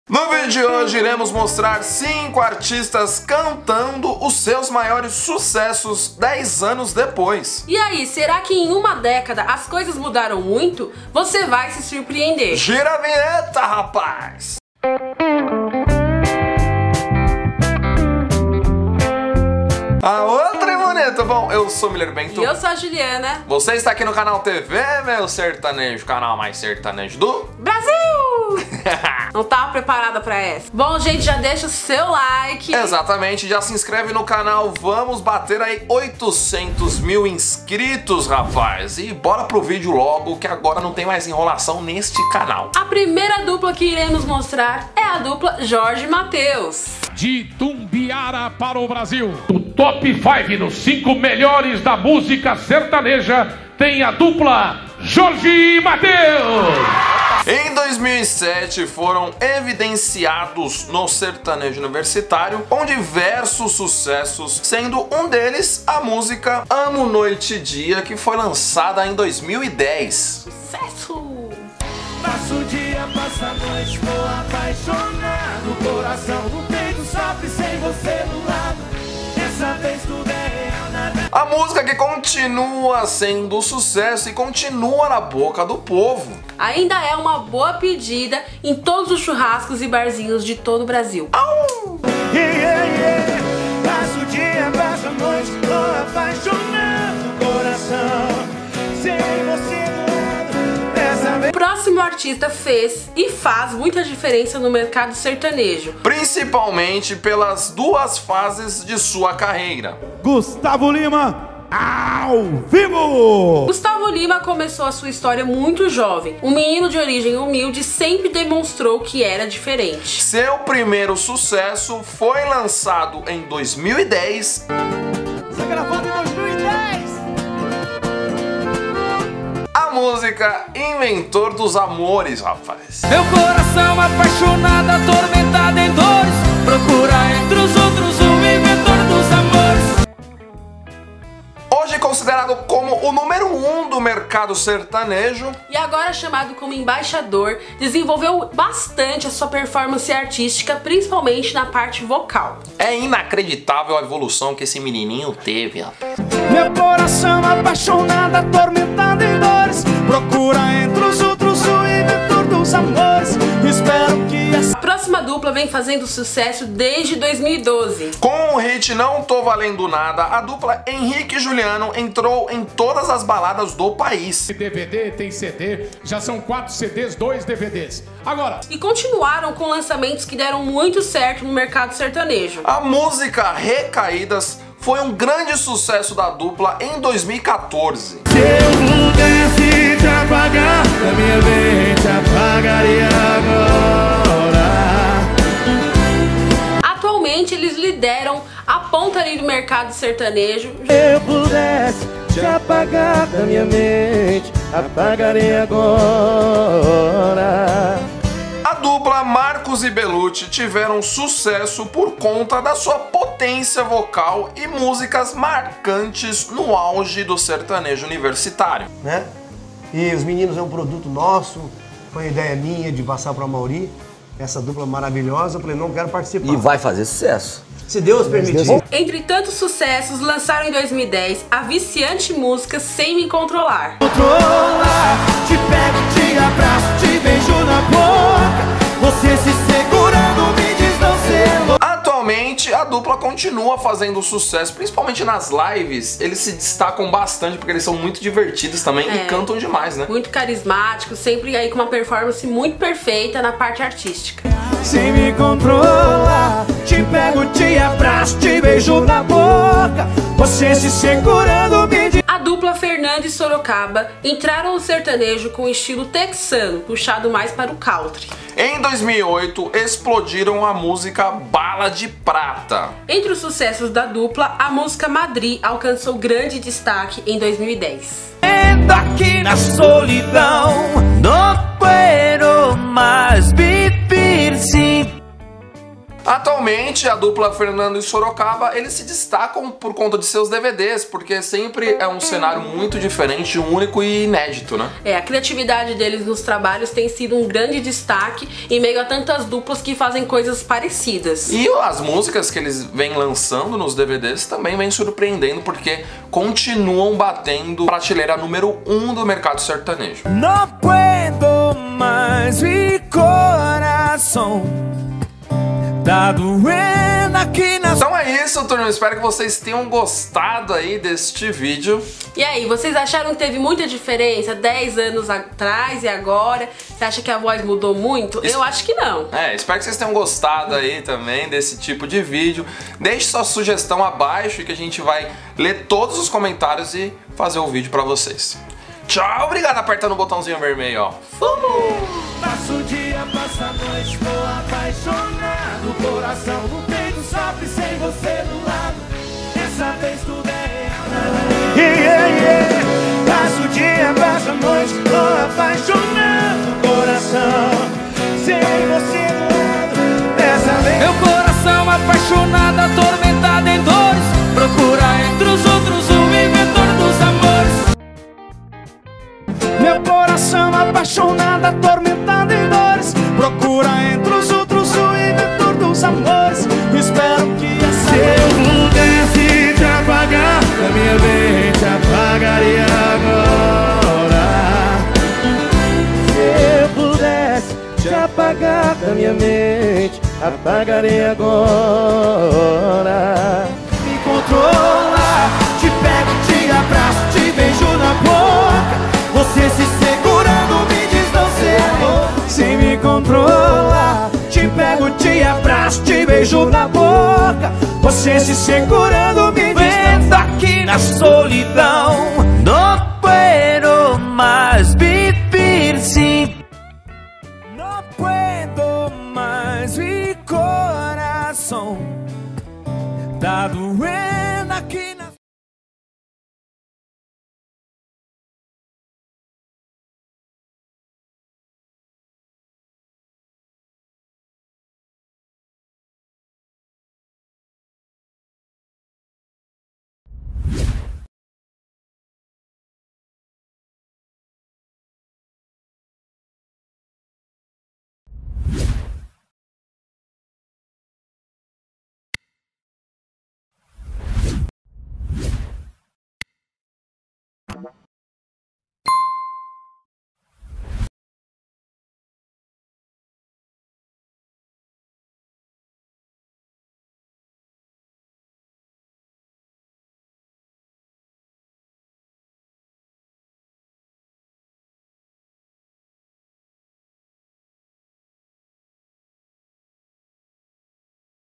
ARTISTAS CANTANDO SEUS SUCESSOS 10 ANOS DEPOIS!
Vídeo de hoje apresento pra vocês cantores e duplas famosos cantando seus hits, seus sucessos mais de 10 anos depois!
Espero que vocês gostem e deixem sugestão para próximos videos.

Parcerias do canal:
Black West: